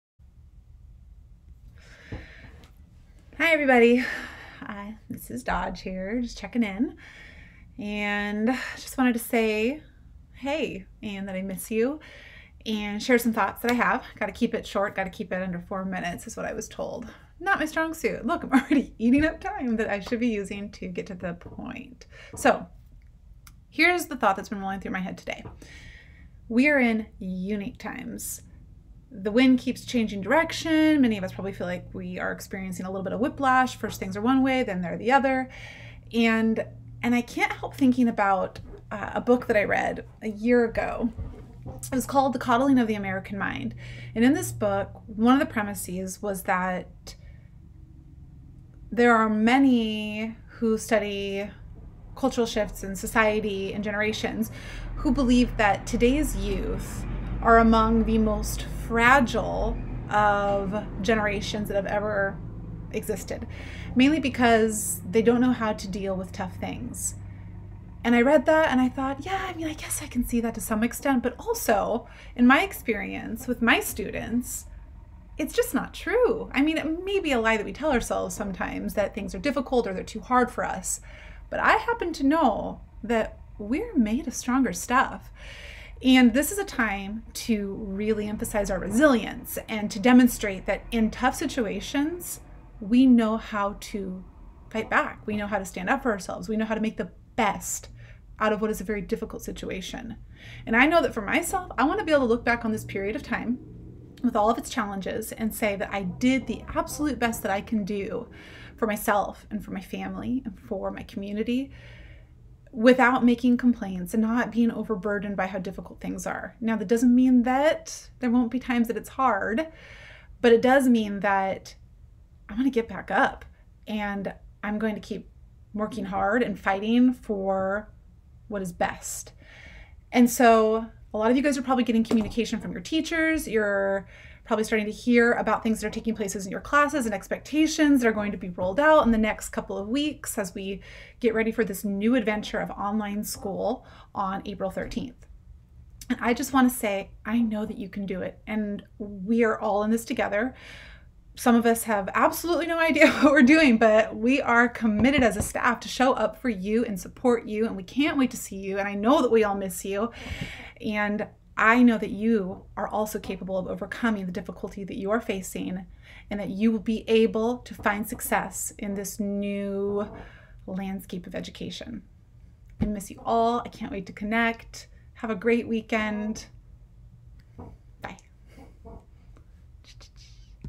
Hello from Mrs Dodge We are RESILIENT!
In tough times, it is easy to feel overburdened. BUT, I am here to say that we are RESILIENT! We are going to band together as a school, and we are going to overcome all of the new challenges in our way as we move toward distance learning as a district. I cannot wait to connect with all of you!!
Hugs & Good Vibes,
Mrs. Dodge :)